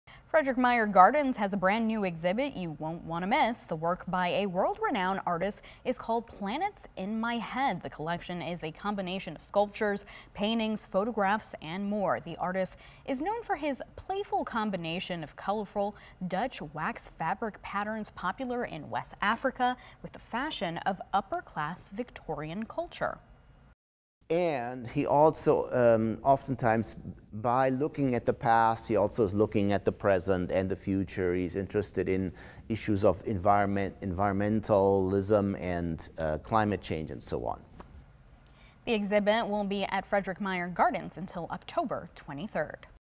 New exhibit comes to Frederik Meijer Gardens
Frederik Meijer Gardens has unveiled a new exhibit you won’t want to miss.

The exhibit highlights work by world-renowned artist Yinka Shonibare, including sculptures, paintings, photographs and more. Called “Planets In My Head,” the display features some of Shonibare’s work from the past three decades.

For assistant curator Jochen Wierich, this exhibit is special. He says it’s one of the first sculpture exhibits the museum has hosted in a long time, as the sculpture galleries have been under renovation.